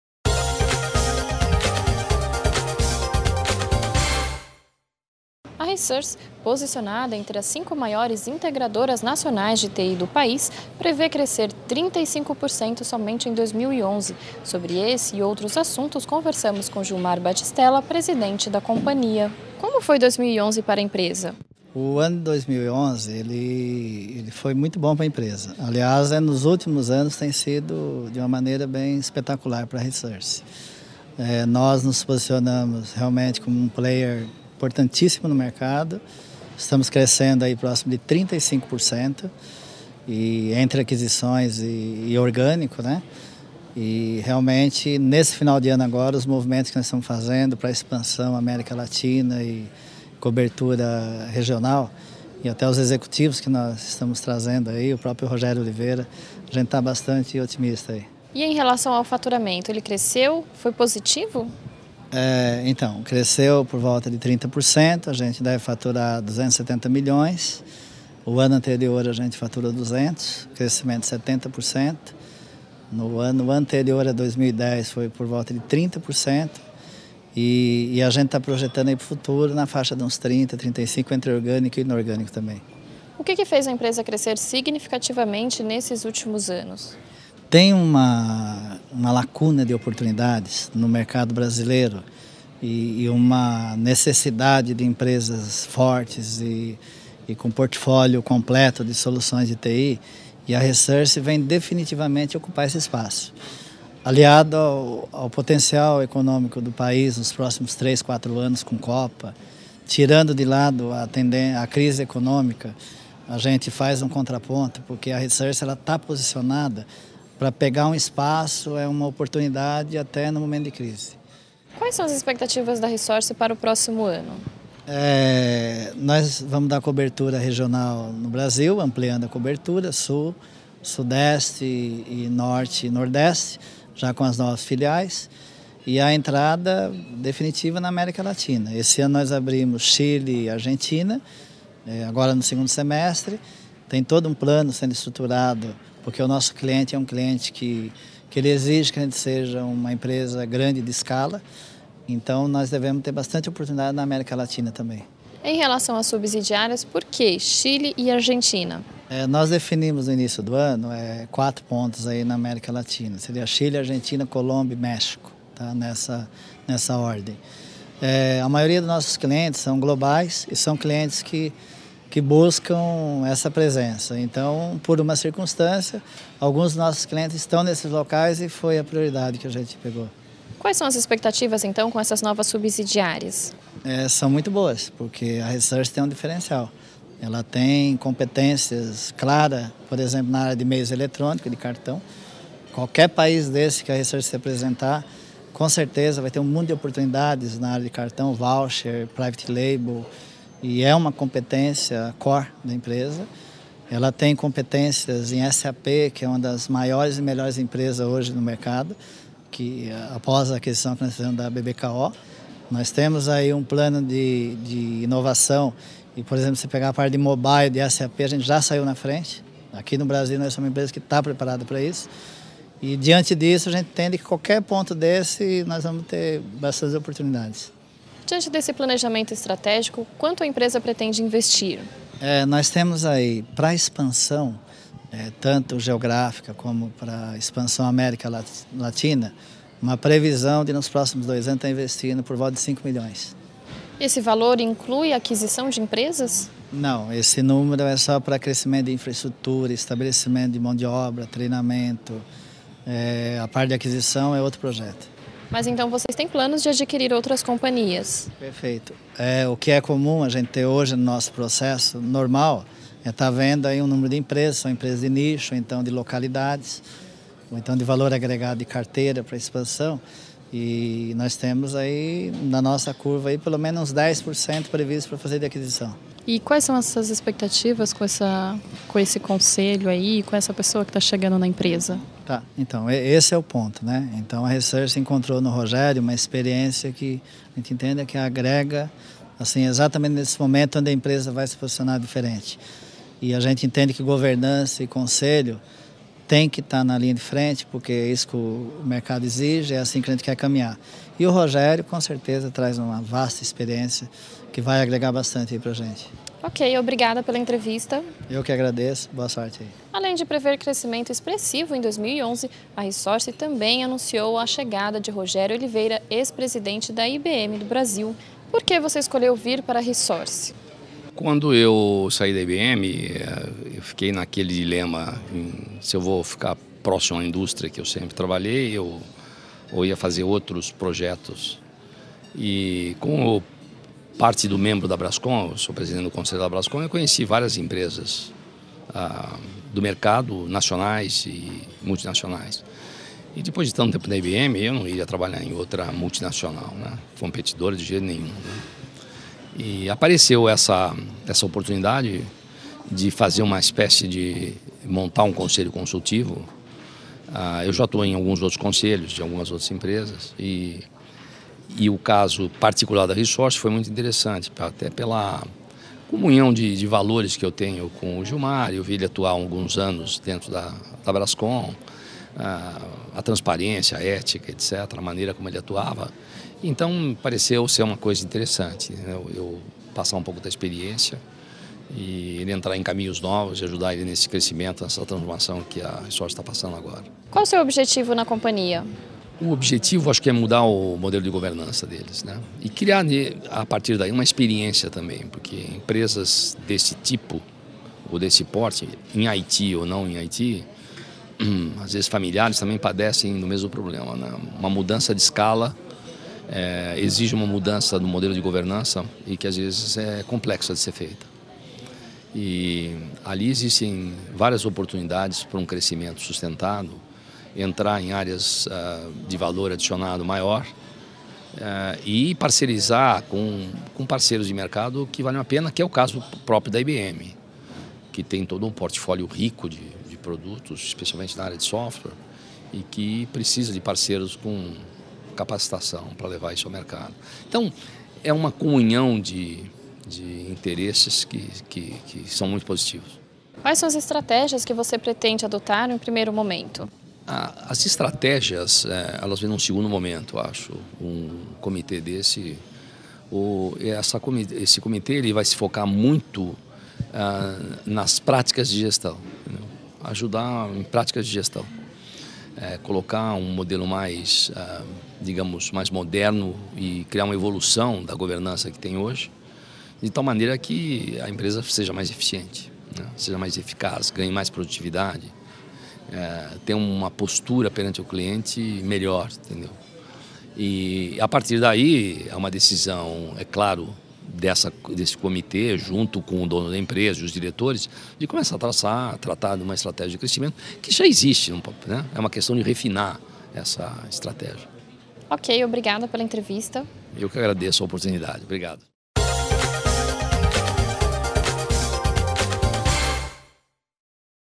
Gilmar Batistela e Rogério Oliveira - Resource - 25/11/11
Entrevista com Gilmar Batistela, presidente da Resource e Rogério Oliveira, ex-presidente da IBM Brasil.